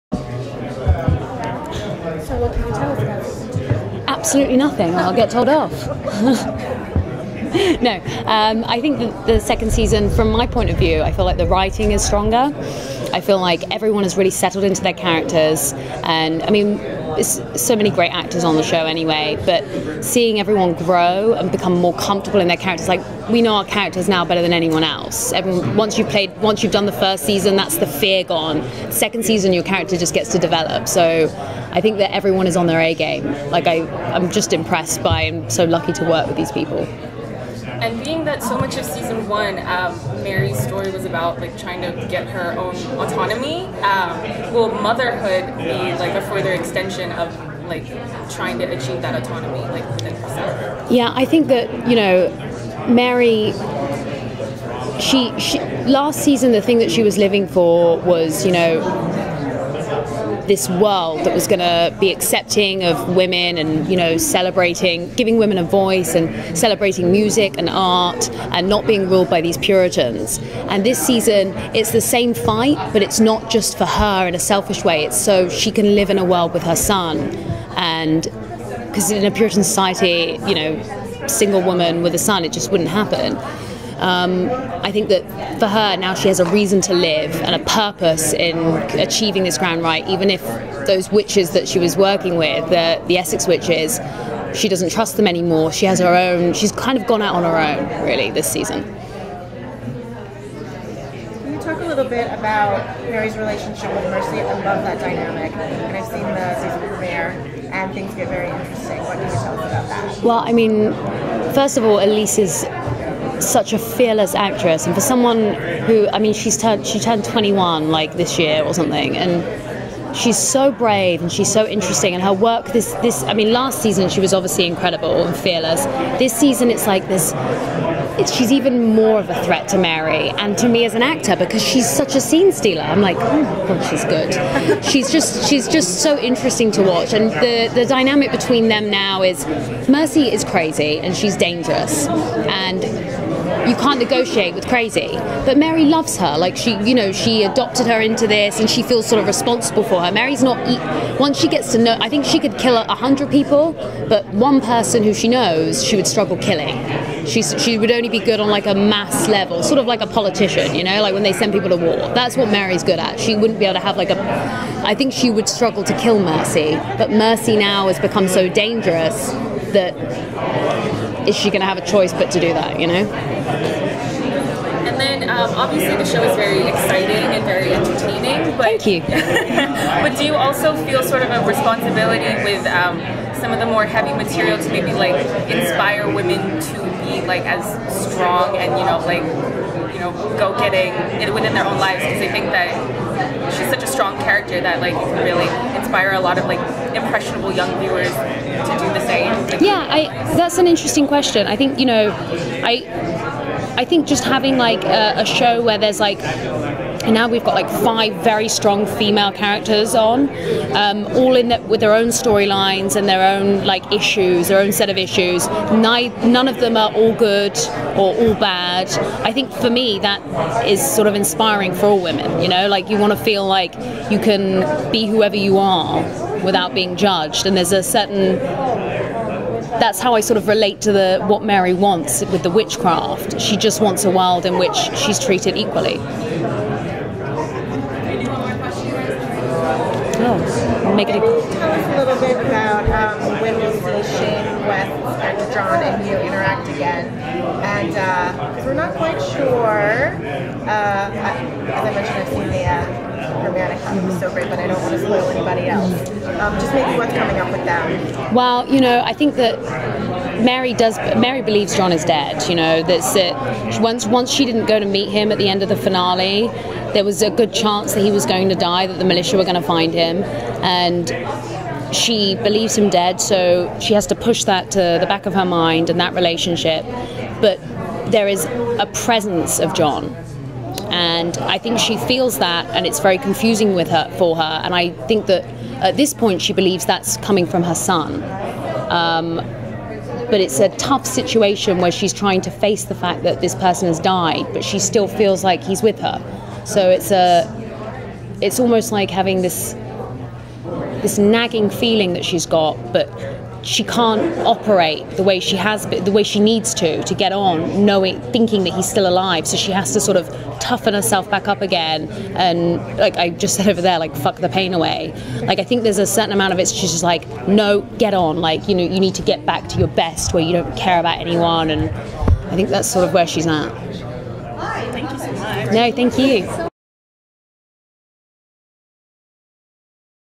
Janet Montgomery talks Salem at Wondercon 2015
Janet Montgomery sits down for a chat in the Wondercon 2015 press room. She talks Mary's path, motherhood, and Mercy.